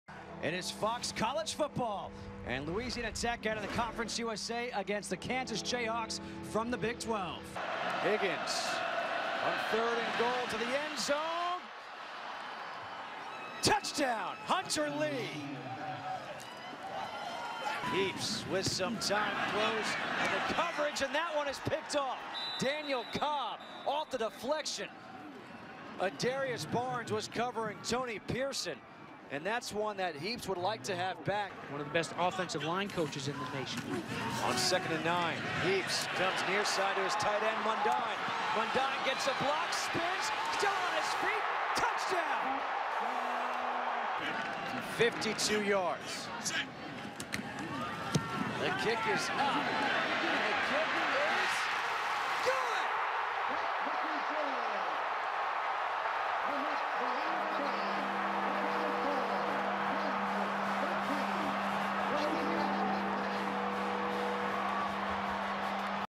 09/21/2013 LA Tech vs Kansas Football Highlights
Jake Heaps passed for 279 yards and a touchdown to lead Kansas to a 13-10 home win over Louisiana Tech.